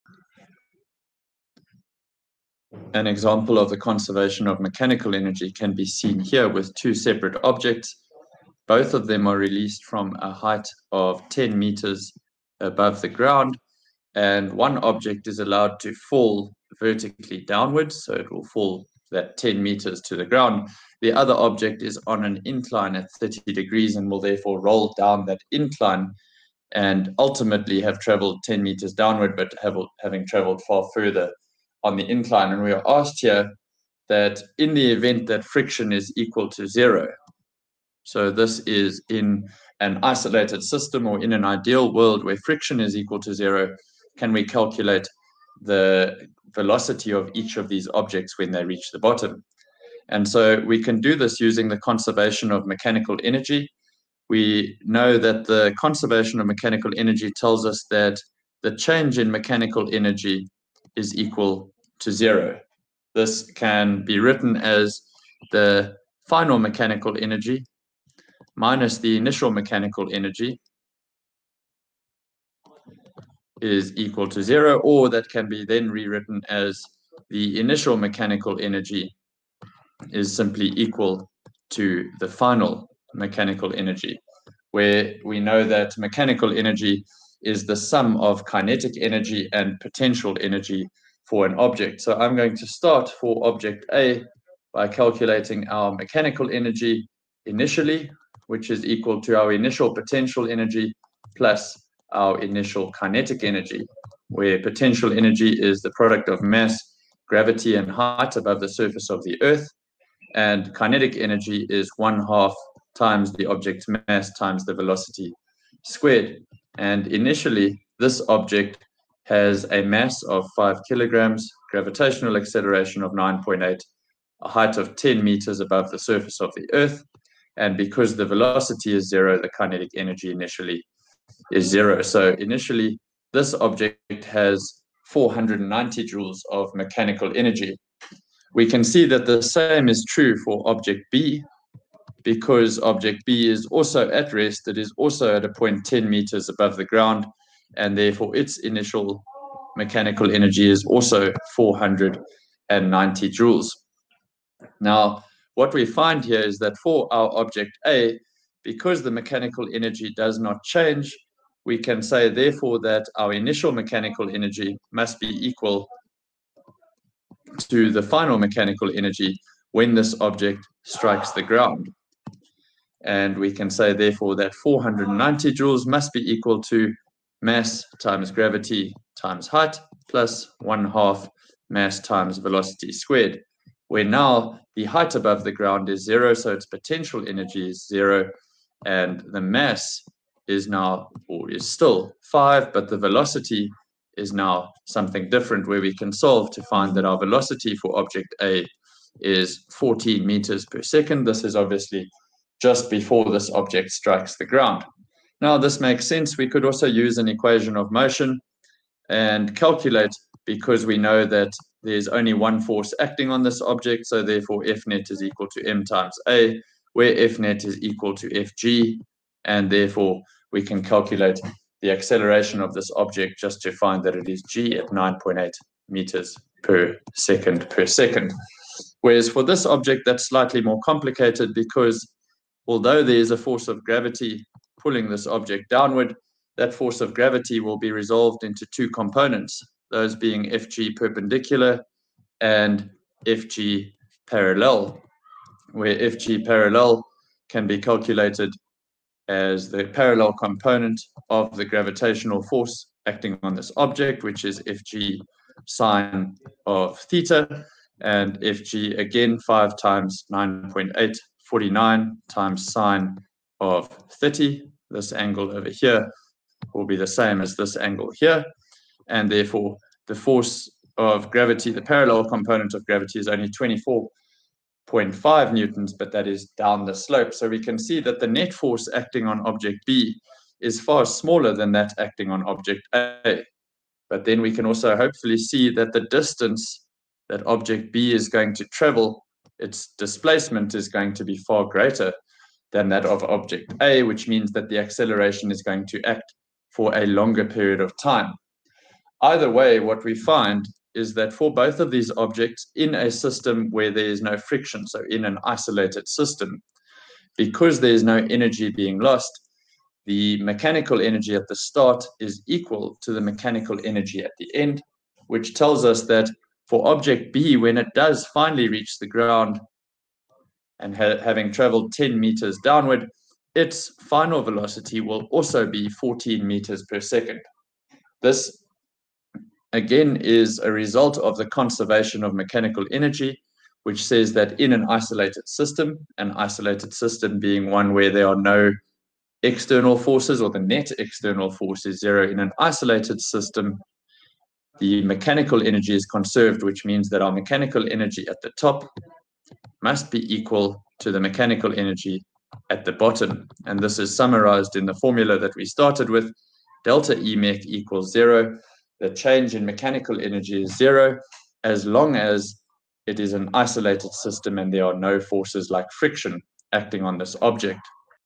Example: Conservation of Energy
Two balls of mass 5 kg are released from rest from a height 10 m above the ground. One object falls vertically downwards and the other rolls down a 30° incline – both in the absence of friction. Calculate the speed with which each object strikes the ground.

South Africa
CAPS
Grade 12
Work, Energy & Power